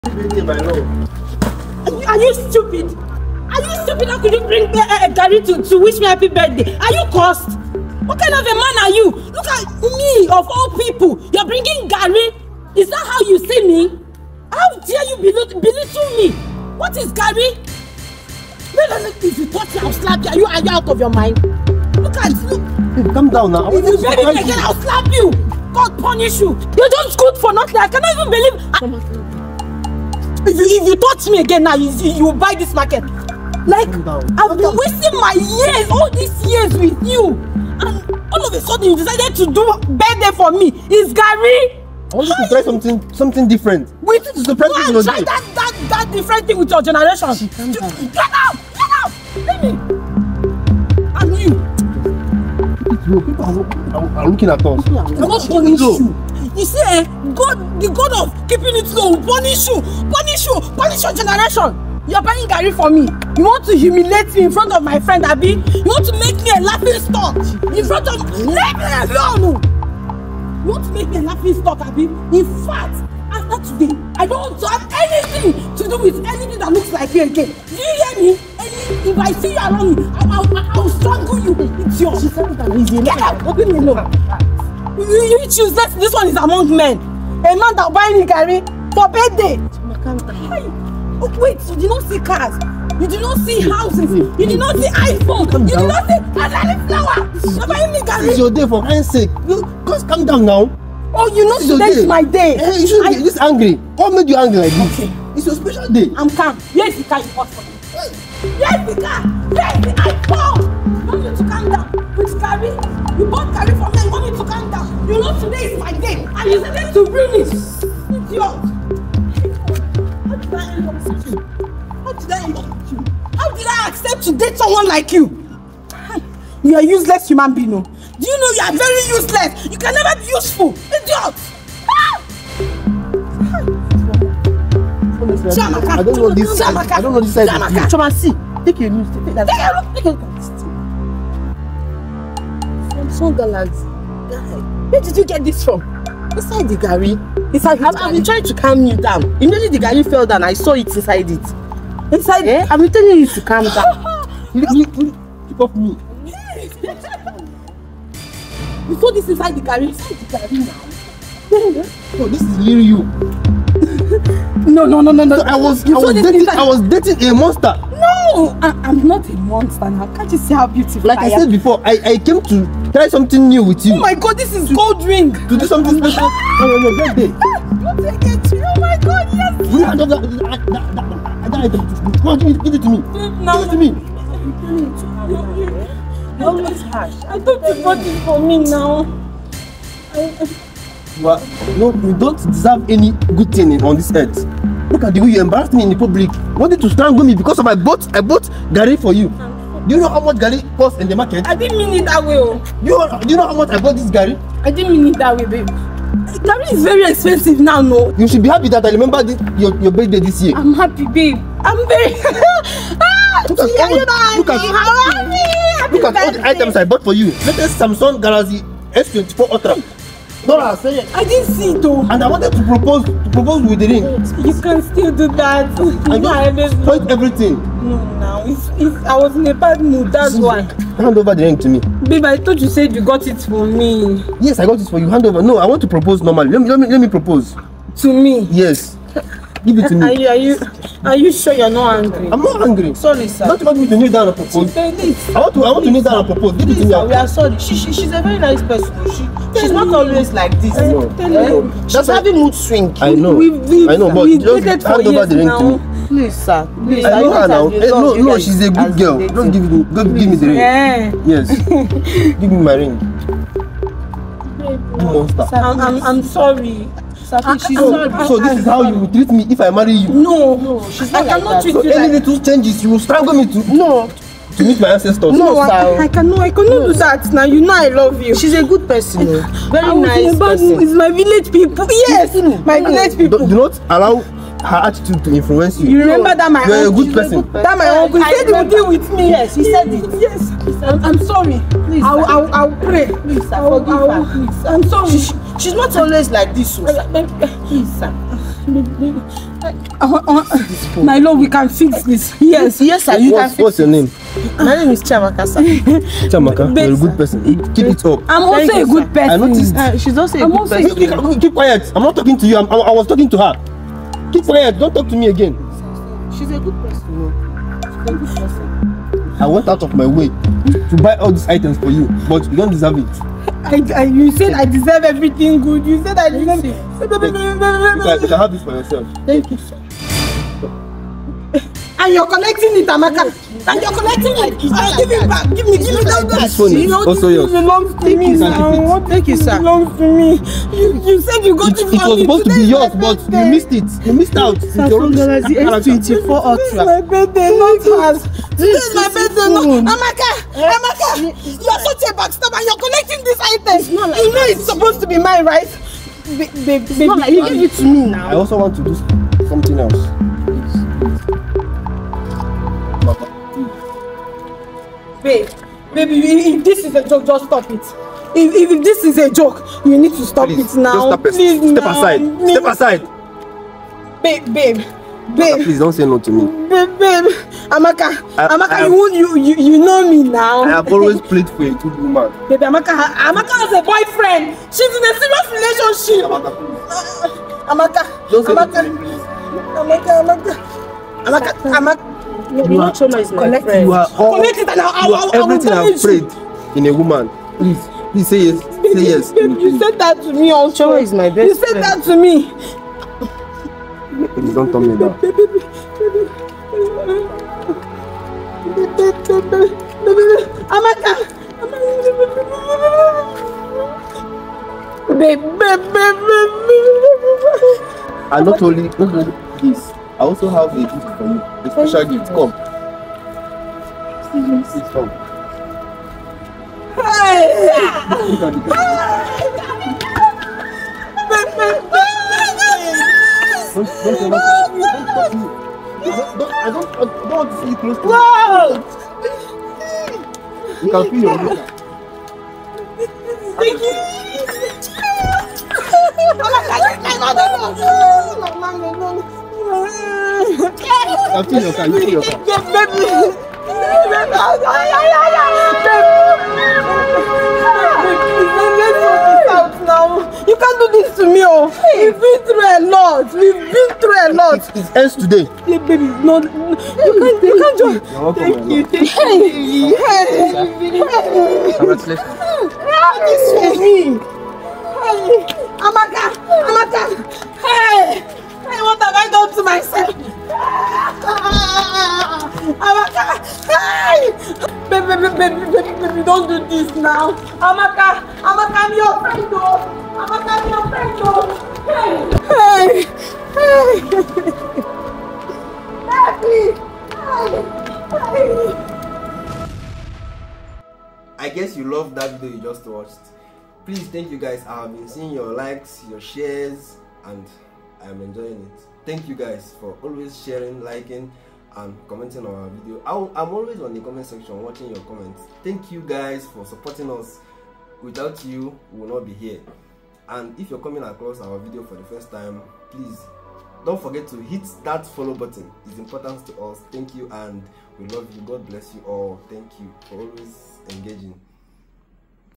THE LOST OPPORTUNITY: Boyfriend's Hidden Gifts Expose Girlfriend's True Colors"
"A shocking tale of rejection, regret, and redemption. Will he take back his insulting girlfriend or find love elsewhere?"